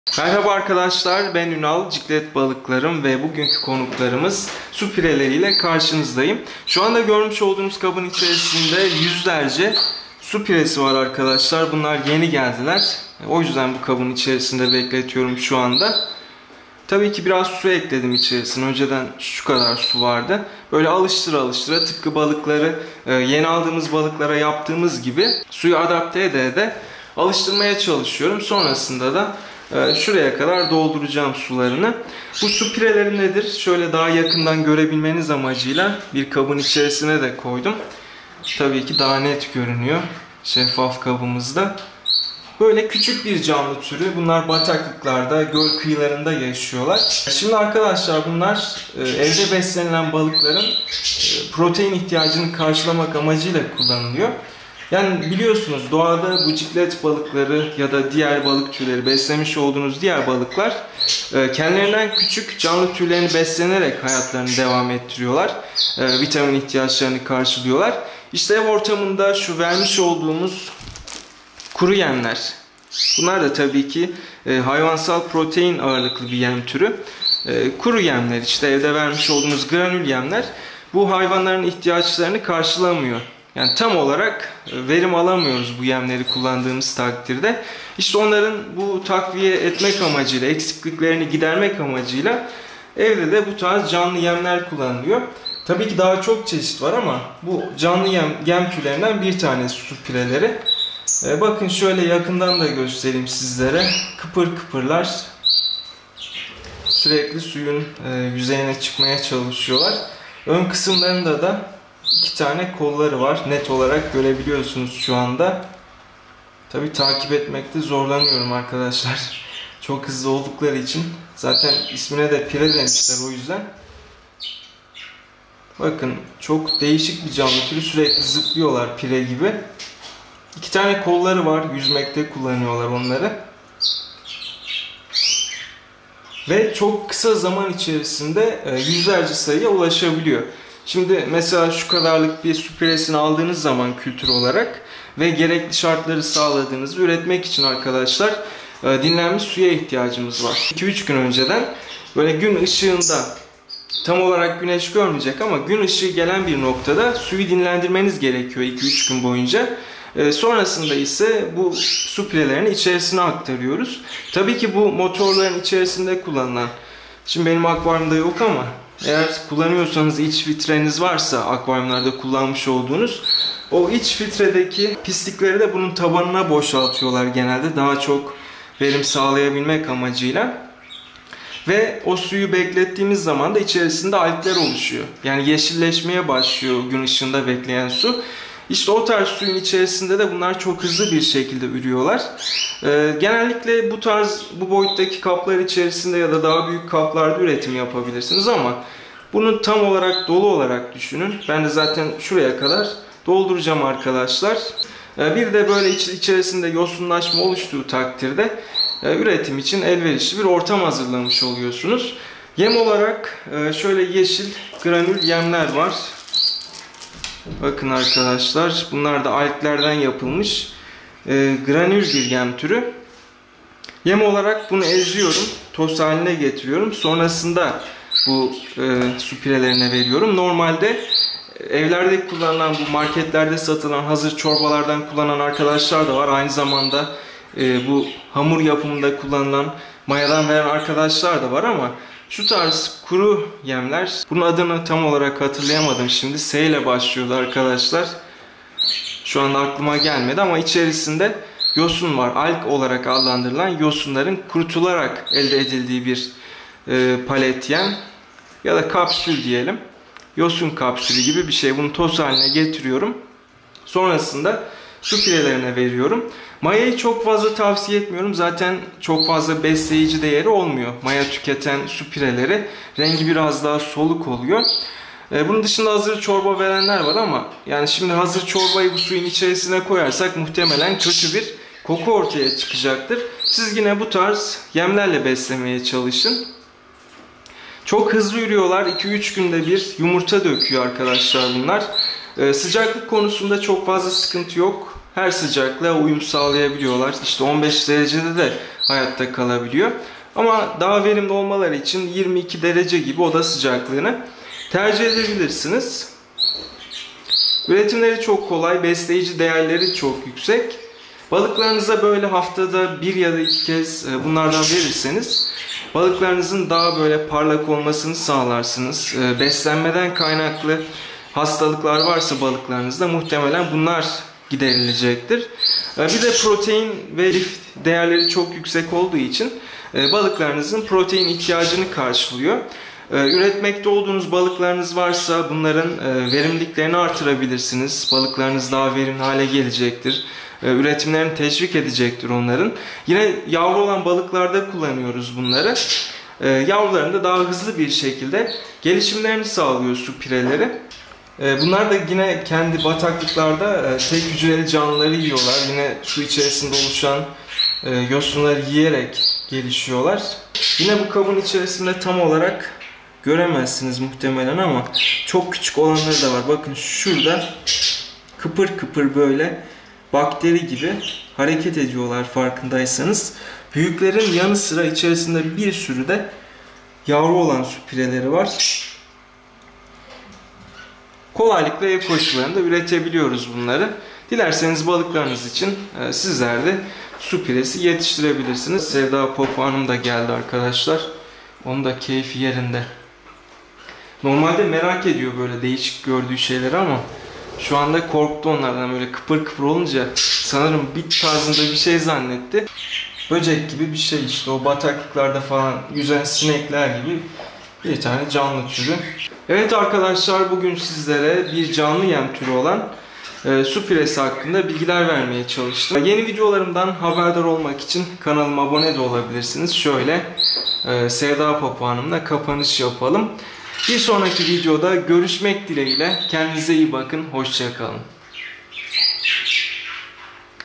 Su Piresi Üretimi | Sarı Prenses Yunus Ciklet Akvaryumu
Bugün ciklet balıkları , canlı doğuran balıkları ve diğer balık türleri için son derece faydalı olan , yavru balık gelişimini destekleyen su pirelerini tanıtmaya çalıştım. Videoyu izleyerek su pirelerinin üretimi, beslenmesi hakkında genel bilgi sahibi olabilirsiniz.